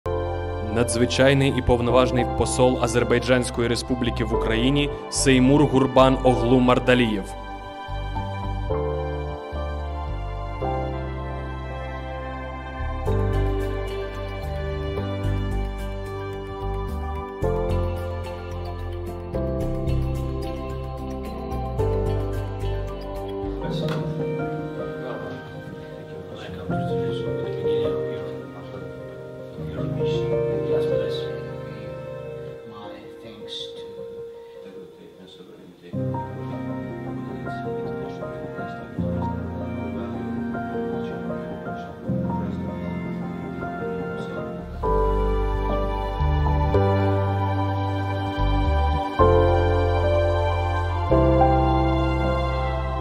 Zelenski Azərbaycana humanitar yardıma görə təşəkkürünü bildirdi
Azərbaycanın Ukraynada yeni təyin olunmuş səfiri Seymur Mərdəliyev etimadnaməsini Prezident Volodimir Zelenskiyə təqdim edib.
Ukraynanın Prezident Ofisinin saytında bildirilir ki, səfir Seymur Mərdəliyevi Ukraynada diplomatik fəaliyyətə başlaması münasibətilə təbrik edən Volodimir Zelenski Azərbaycanın Ukraynanın ərazi bütövlüyü və suverenliyinə verdiyi dəstəyə görə minnətdarlığını bildirib.
Volodimir Zelenski Azərbaycan Prezidenti İlham Əliyevə ən səmimi təbriklərini çatdırıb və onların Kişineudakı son görüşlərinin məhsuldar keçdiyini, qarşılıqlı maraq doğuran bir çox məsələləri müzakirə edə bildiklərini qeyd edib: “Xüsusilə, biz Azərbaycanın İrpen şəhərinin bərpasında iştirakına razılaşdıq”.
“Bizim komandalarımız artıq bu sazişin həyata keçirilməsi üzərində işləyir. Bu, Ukraynanın müharibədən sonrakı bərpasına konkret töhfə olacaq”, – deyə dövlət başçısı vurğulayıb.